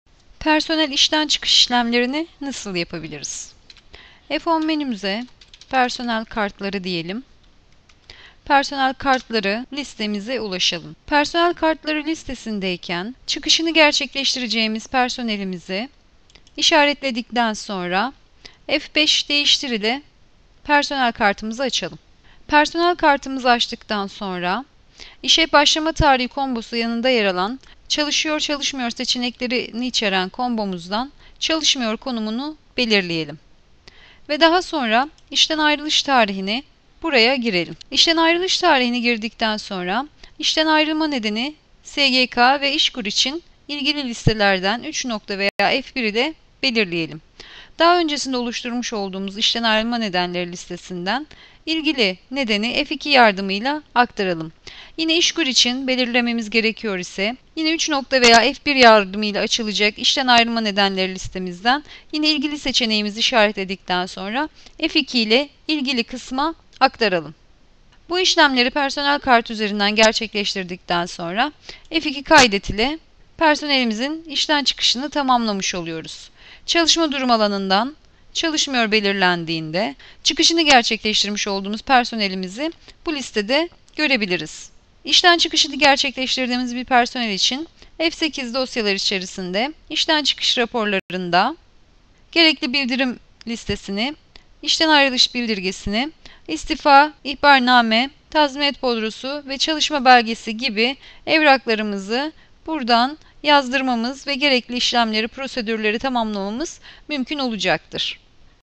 Personel İşten Çıkış İşlemlerini Nasıl Yapabilirim?
Dia personel modülünde personel işten çıkış işlemini nasıl yapılacağını anlatmaktadır.